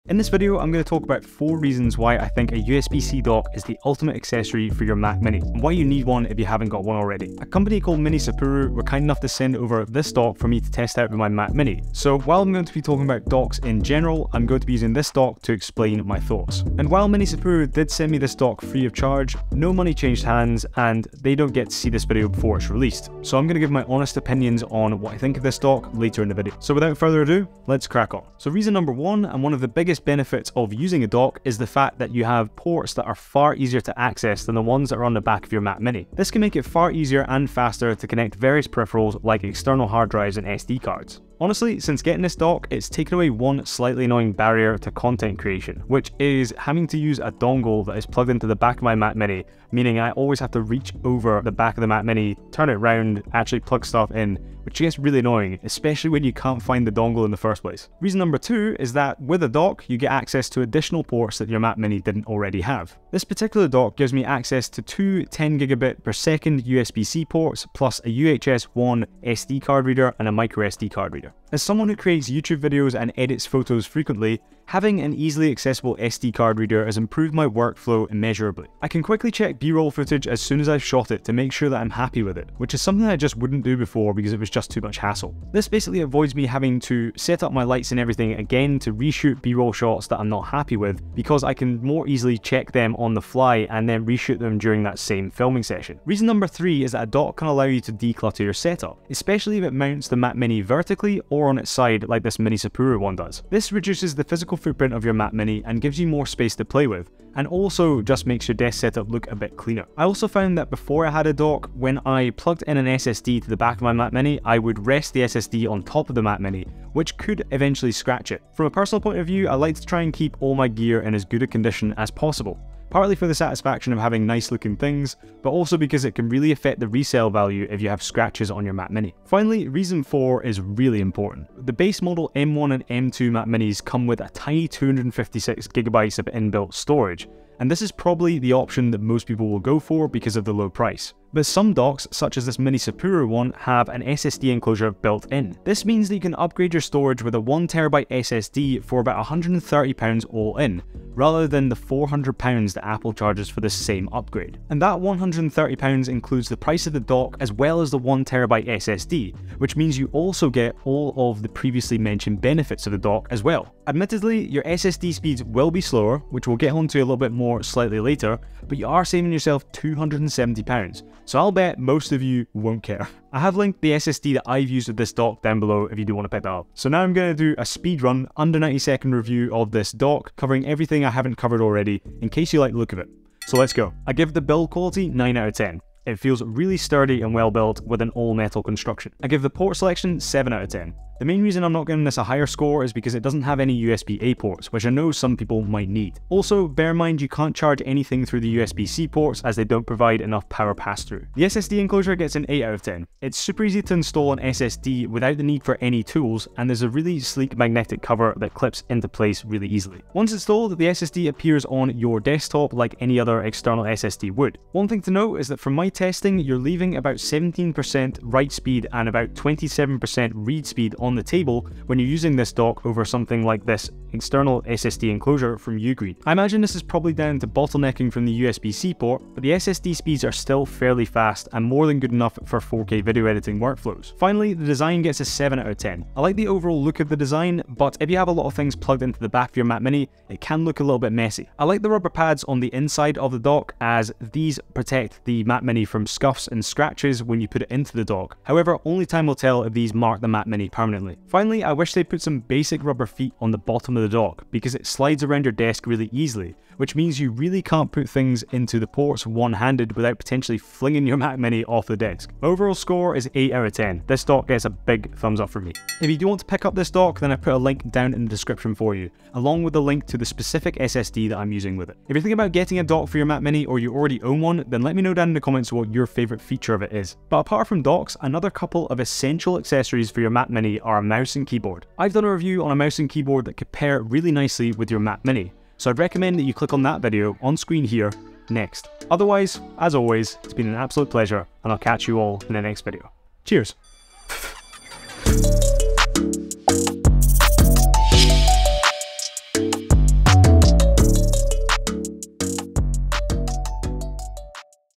The ULTIMATE Accessory for Your Mac Mini!
In this video, I discuss the benefits of using a USB-C dock with a Mac mini, specifically focusing on a dock from a company called Minisopuru. I give you four reasons why a dock is an essential accessory for your Mac mini, including easier access to ports, access to additional ports, decluttering your setup, and the ability to upgrade storage at a lower cost. I also do a mini review of the Minisopuru dock, covering build quality, port selection, SSD enclosure, and design.

Minisopuru Mac mini Dock
WD SN570 1TB SSD
UGREEN SSD Enclosure
M2 Mac Mini